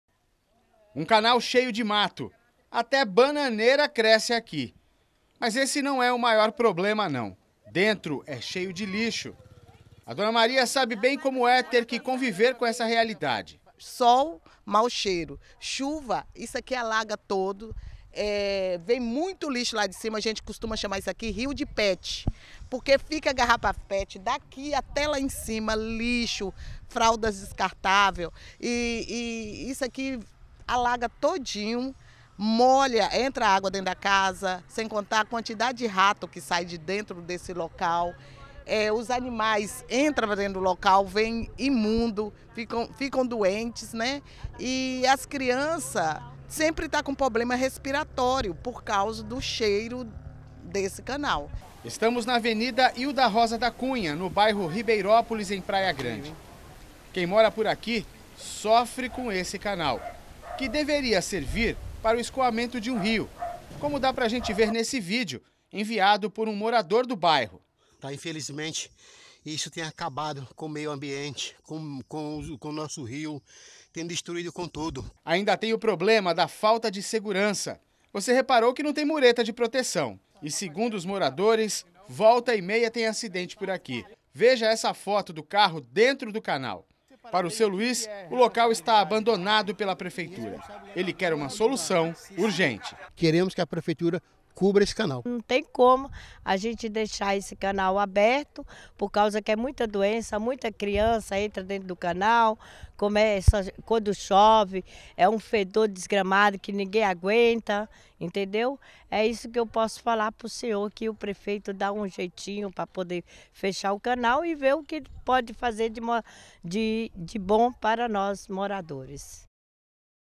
Um canal vem “tirando o sono” dos moradores de um bairro em Praia Grande
Um canal vem “tirando o sono” dos moradores de um bairro em Praia Grande. O estado de abandono, causa inúmeros problemas para quem vive no local.